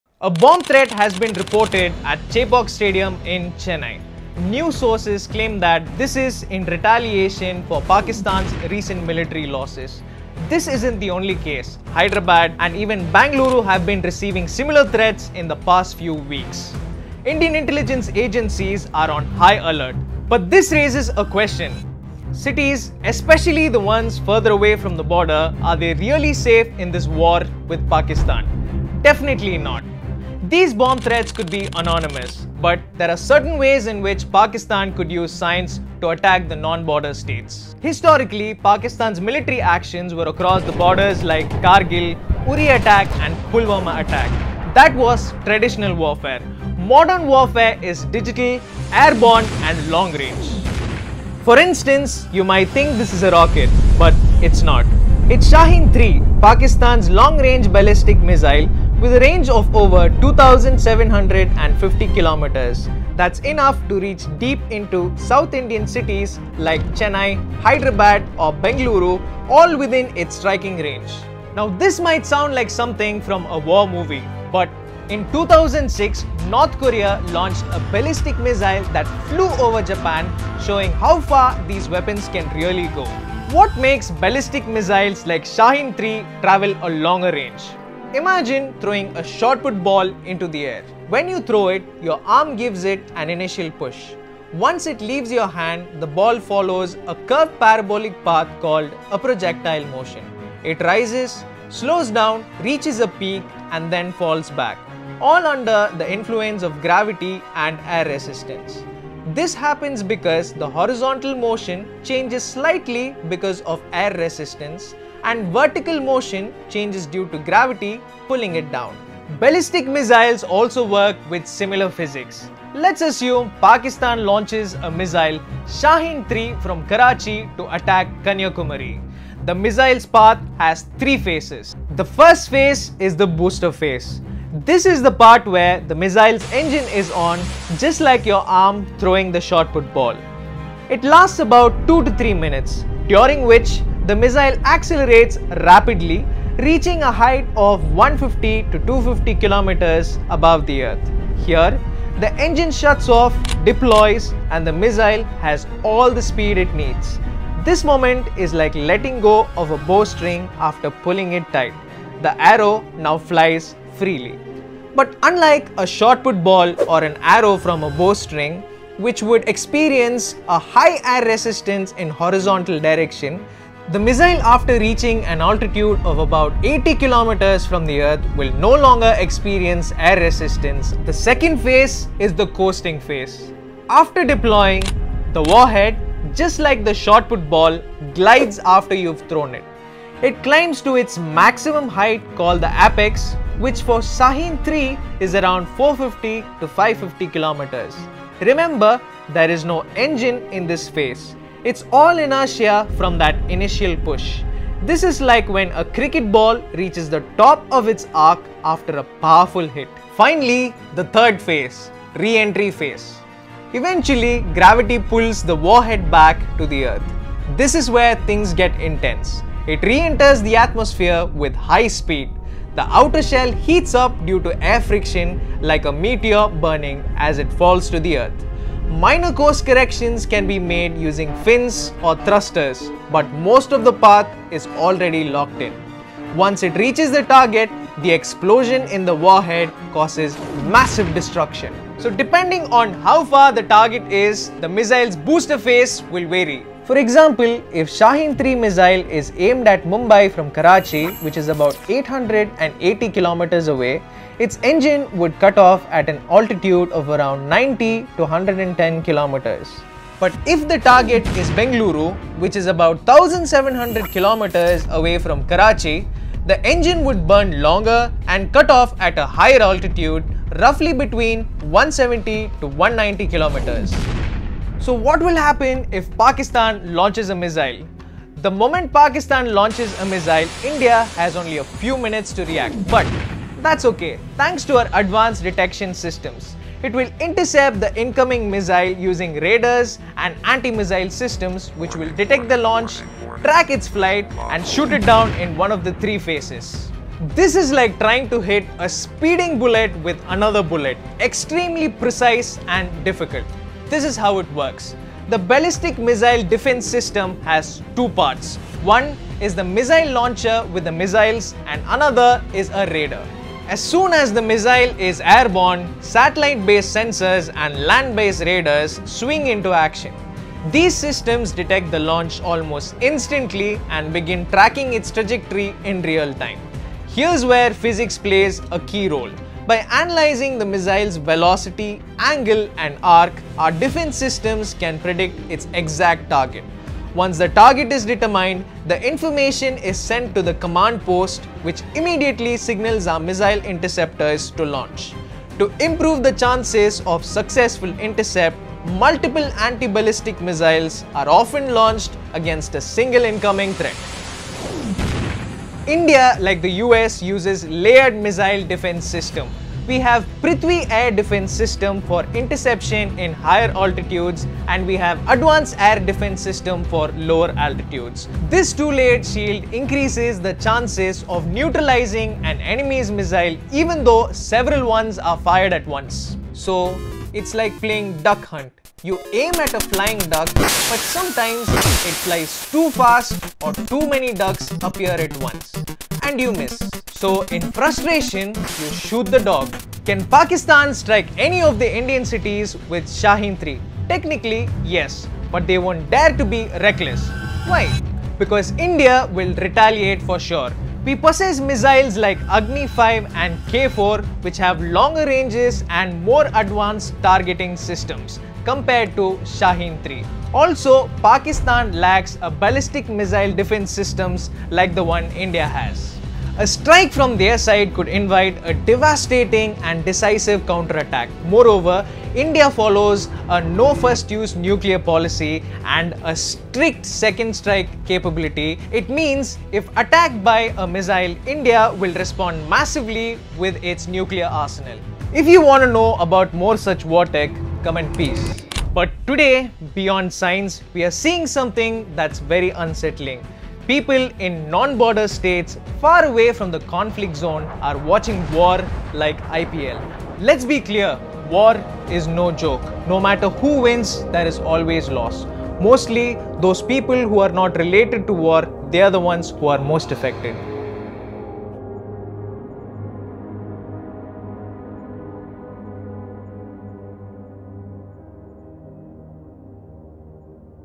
Pakistan Ballistic Missile vs Indian Defence | Chitti Global
With recent commotion and growing tensions, this video " How Ballistic Missile works ? "  dives deep into the science behind modern warfare — how ballistic missiles like Shaheen-III work, how India’s missile defense system (PAD & AAD) intercepts them, and why non-border cities are no longer safe in today's digital, airborne wars.

From Ballistic Missile working physics to real-time interception, this is a full breakdown of how modern warfare unfolds in seconds, backed by satellite tracking, radar systems, and scientific precision with Animation.

Pakistan is a threat - 0:00
Traditional war vs modern warfare - 0:36
Shaheen 3 - 0:50
North korea's ballistic missile -1:11
Working of ballistic missile - 1:22
Boaster phase - 2:09
Coasting phase - 3:00
Re -entry phase - 3:18
Shaheen 3 strike - 4:02
Indian defence system - 4:35
Defence system working - 5:05
Ind vs Pak Military comparison - 6:16
Peace - 6:58
Unsettling truth - 7:04